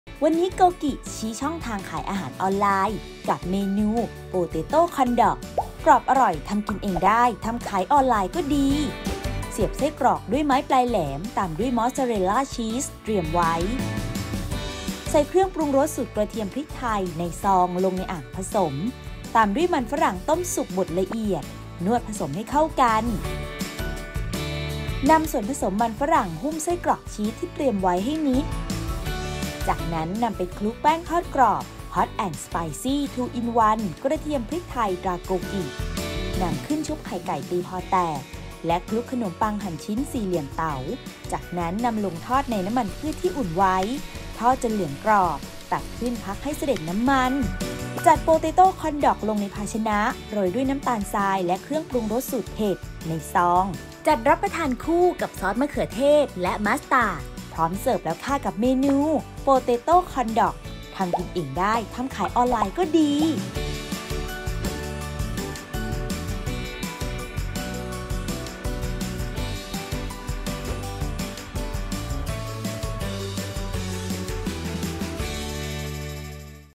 โปเตโต้คอร์นด็อก สูตรเด็ด ทำง่าย สไตล์ "อ.ยิ่งศักดิ์"
โปเตโต้ คอร์นด็อก (ผลิตภัณฑ์แป้งทอดกรอบ ฮอทแอนด์สไปซี่ 2 in1 กระเทียมพริกไทย ตราโกกิ )

ไส้กรอกหั่นครึ่ง                                   4         ชิ้น
มอสเซอเรล่าชีสหั่นขนาดเท่าไส้กรอก    4        ชิ้น
ไม้เสียบปลายแหลม                            4        อัน
เครื่องปรุงรสสูตรกระเทียมพริกไทย (ในซอง)   1   ซอง
มันฝรั่งต้มสุกบดละเอียด                     300    กรัม
แป้งทอดกรอบ ฮอทแอนด์สไปซี่ 2 in1 กระเทียมพริกไทย ตราโกกิ                                                    100     กรัม
ขนมปังหั่นสี่เหลี่ยมเต๋า สำหรับคลุก
น้ำตาลทรายและเครื่องปรุงรสสูตรเผ็ด (ในซอง) สำหรับโรยหน้า
ซอสมะเขือเทศและมัสตาร์ด สำหรับรับประทานคู่

1. เสียบไส้กรอกด้วยไม้ปลายแหลม ตามด้วยมอสเซอเรล่าชีส เตรียมไว้
2. ใส่เครื่องปรุงรสสูตรกระเทียมพริกไทย (ในซอง)ลงในอ่างผสม ตามด้วยมันฝรั่งต้มสุกบดละเอียด นวดผสมให้เข้ากัน
3. แบ่งส่วนผสมเป็นก้อน ก้อนละ 50 กรัม เตรียมไว้
4. นำส่วนผสมมันฝรั่งหุ้มไส้กรอกชีสที่เตรียมไว้ให้มิด จากนำไปคลุกแป้งทอดกรอบ ฮอทแอนด์สไปซี่ 2 in1 กระเทียมพริกไทย ตราโกกิ
5. นำขึ้นชุบไข่ไก่ตีพอแตก และคลุกขนมปังหั่นสี่เหลี่ยมเต๋า จากนั้นนำลงทอดในน้ำมันพืชที่อุ่นไว้ ทอดจนเหลืองกรอบ ตักขึ้น พักให้สะเด็ดน้ำมัน
6. จัดโปเตโต้คอร์นด็อกลงในภาชนะ โรยด้วยน้ำตาลทรายและเครื่องปรุงรสสูตรเผ็ด (ในซอง) จัดรับประทานคู่กับซอสมะเขือเทศและมัสตาร์ด พร้อมเสิร์ฟ

▲Line @yingsakfood